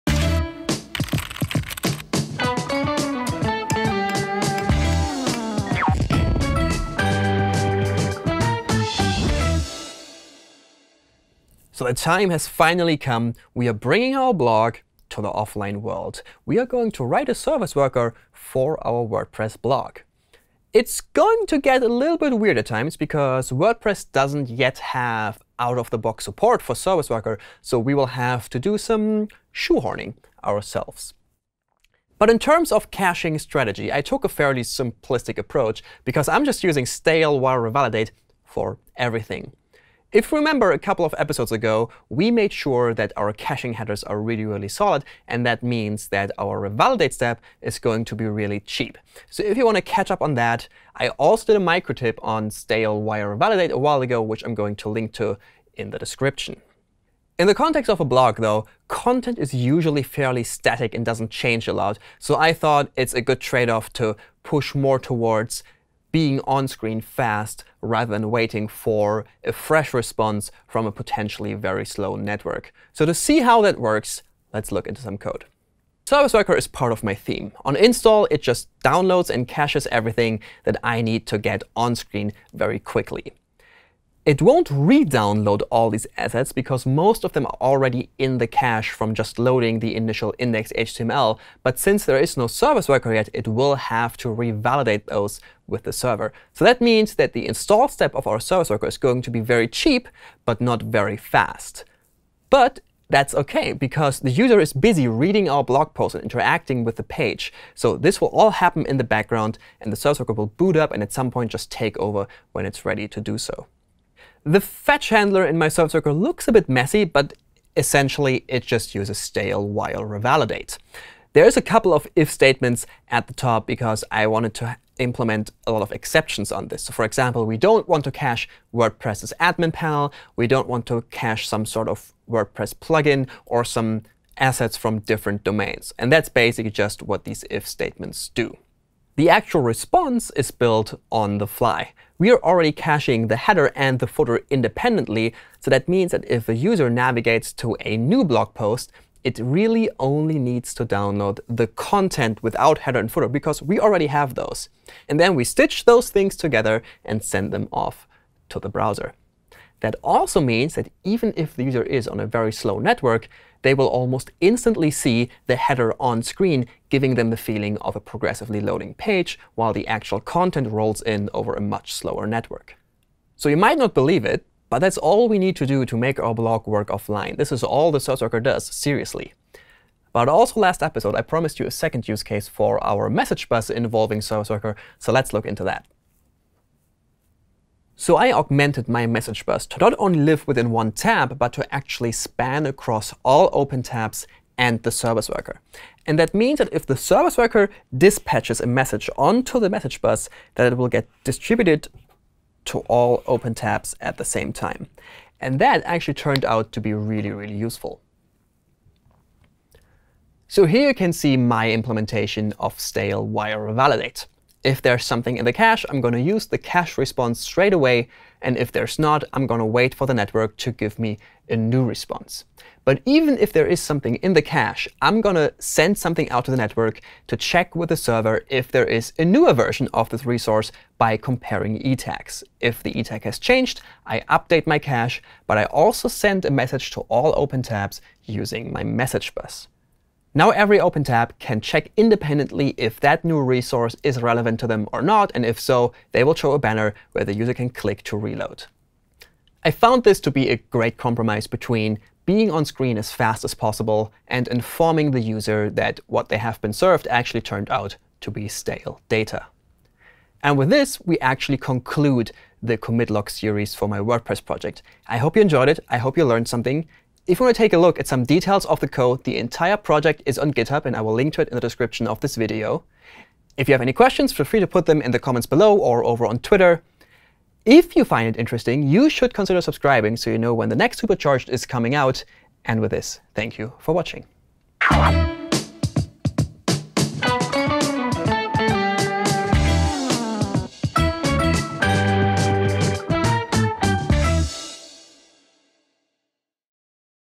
ServiceWorker for WordPress: Commit Log - Supercharged
Surma is building a PWA using WordPress. In this episode he explains how he made the blog work offline.